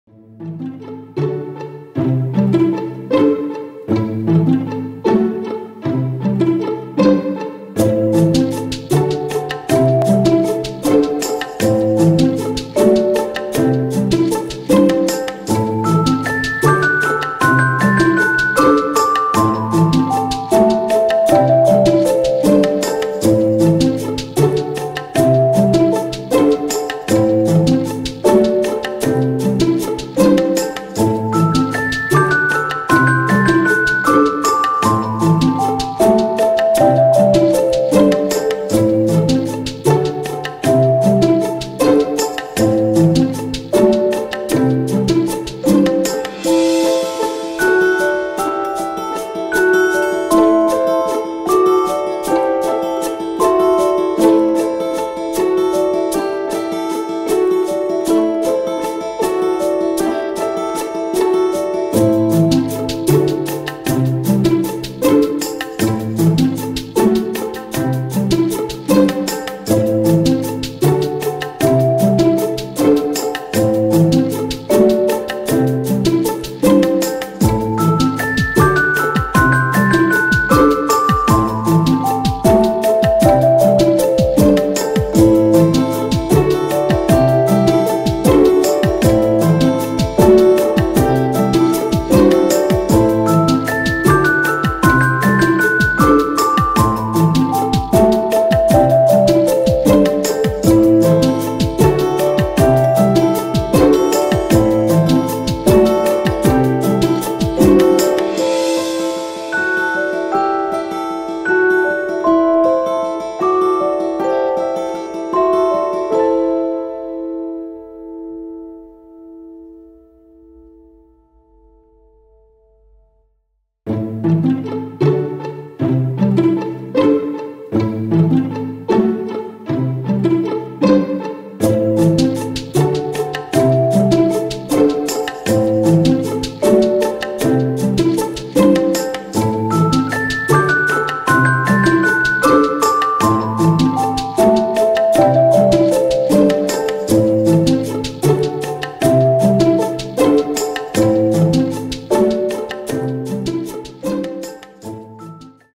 Gulf Breeze Over Strand Street - Galveston
This is a time lapse video showing my painting process for this watercolor on paper.